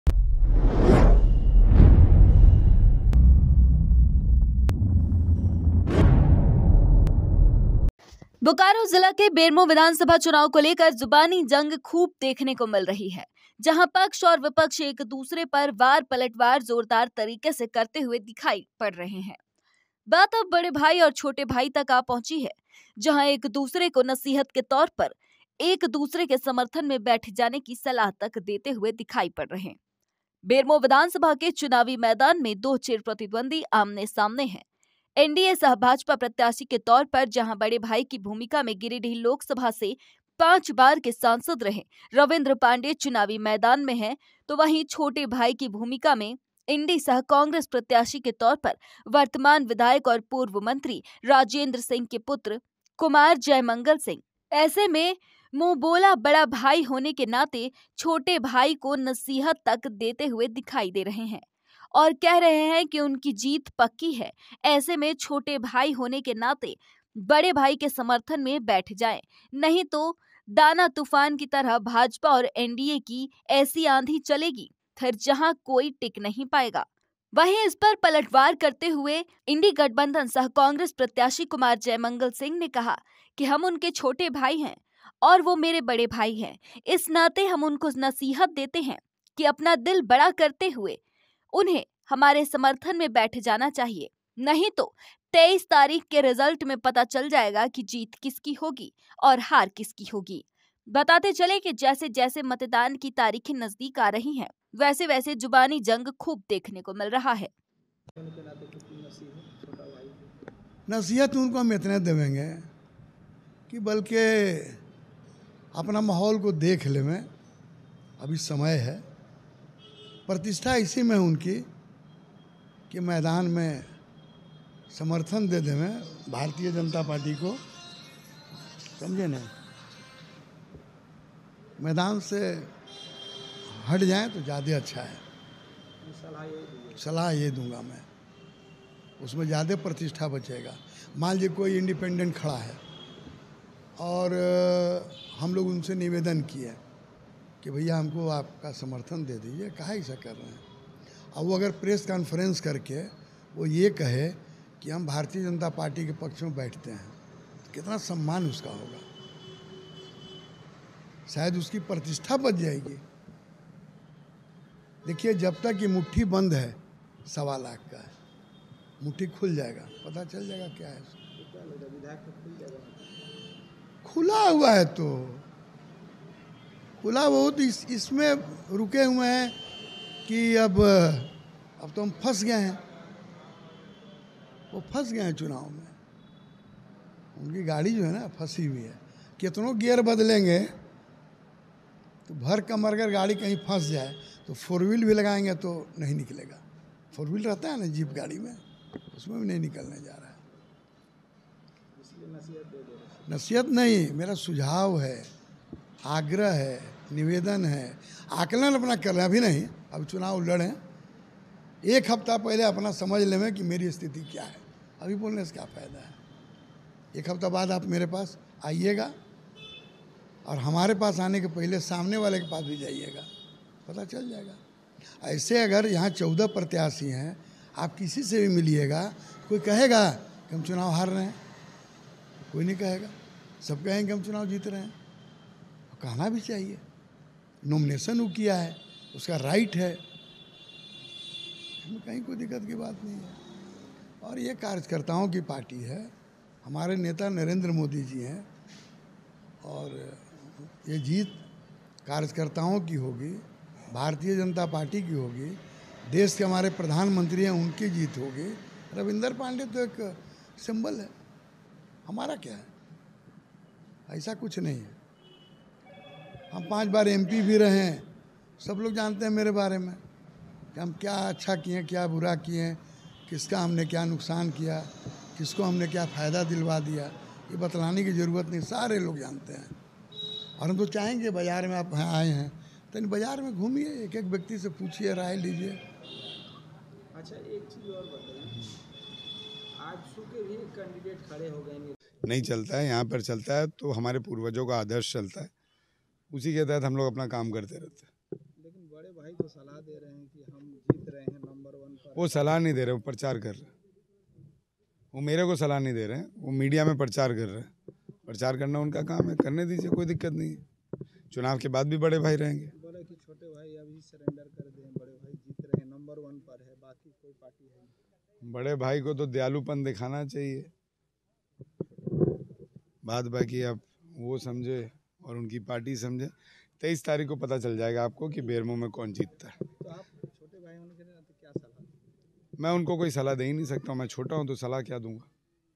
NDA के शरण में आ जाए कांग्रेस प्रत्याशी नहीं तो तूफान में उड़ जाएंगे : रविंद्र पाण्डेय
बोकारो जिला के बेरमो विधानसभा चुनाव को लेकर जुबानी जंग खूब देखने को मिल रही है जहां पक्ष और विपक्ष एक दूसरे पर वार पलटवार जोरदार तरीके से करते हुए दिखाई पड़ रहे है। बात अब बड़े भाई और छोटे भाई तक आ पहुंची है जहां एक दूसरे को नसीहत के तौर पर एक दूसरे के समर्थन में बैठ जाने की सलाह तक देते हुए दिखाई पड़ रहे है।